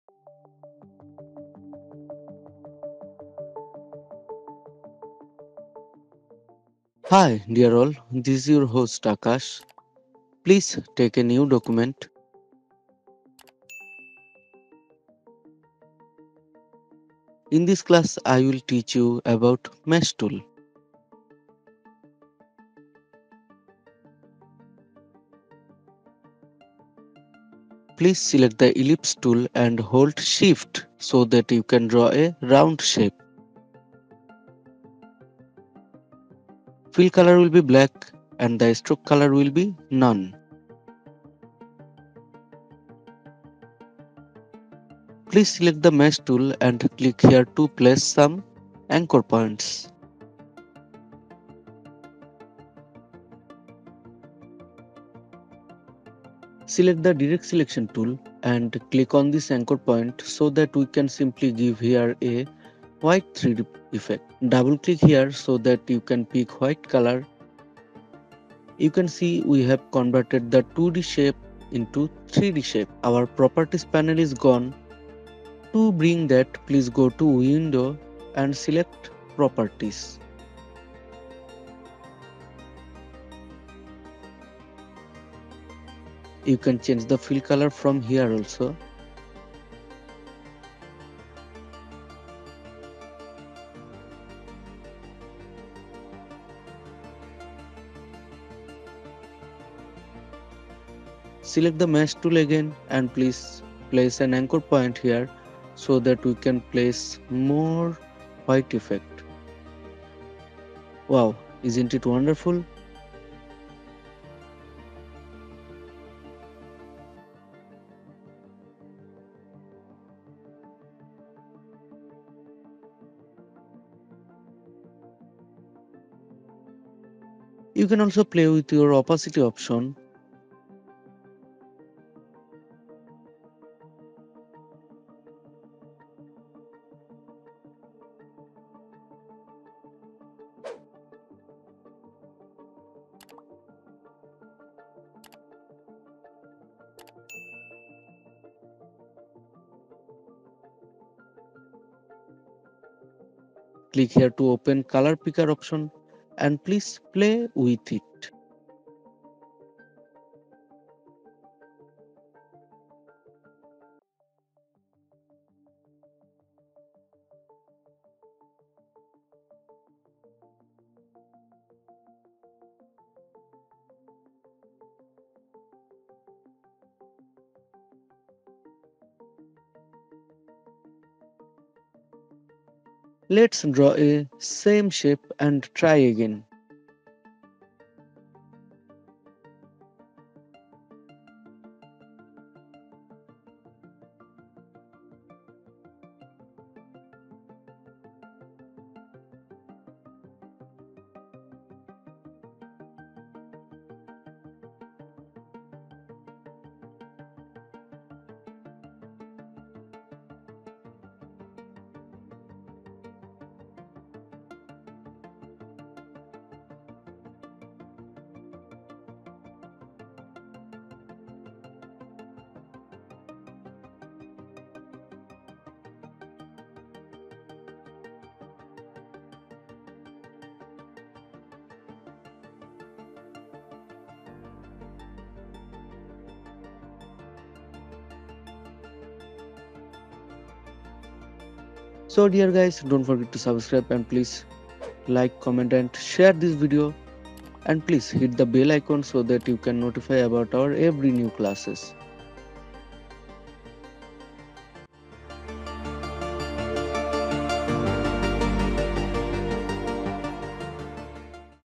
How to use mesh tool in l Adobe Illustrator CC 2022 - Class 20
Hi dear all this is your host Akash.
Please take a new document.
In this class I will teach you about mesh tool.
Please select the ellipse tool and hold shift so that you can draw a round shape.
Fill color will be black and stroke will be none.
Please select the mesh tool and click here to place some anchor points.
Select the direct selection tool and click on this anchor point so that we can simply give here a white 3D effect. Double click here so that you can pick white color. You can see we have converted the 2D shape into 3D shape. Our properties panel is gone to bring that please go to window and select properties.
You can change the fill color from here also.
Select the mesh tool again and please place an anchor point here so that we can place more white effect. Wow isn’t it wonderful?

You can play with opacity option. Click here to open the color picker option and please play with it.

Let’s draw a same shape and try again.